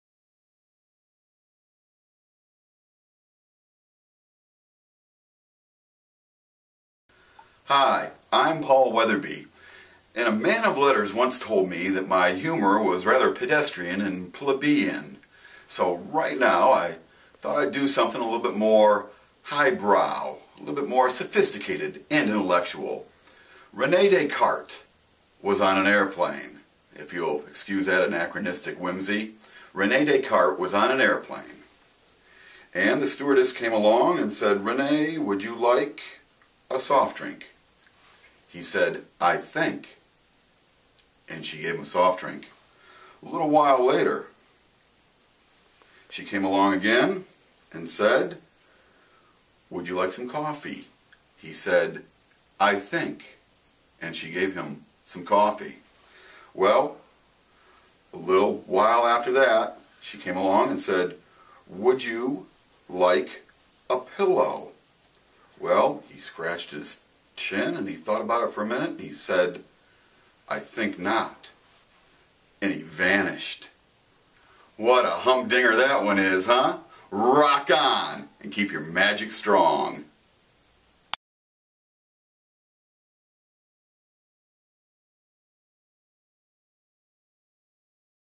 Weatherbee Wednesday: Episode 9 "Descartes"
Paul Weatherbee  does an intellectual trick about the famous philosopher, Descartes.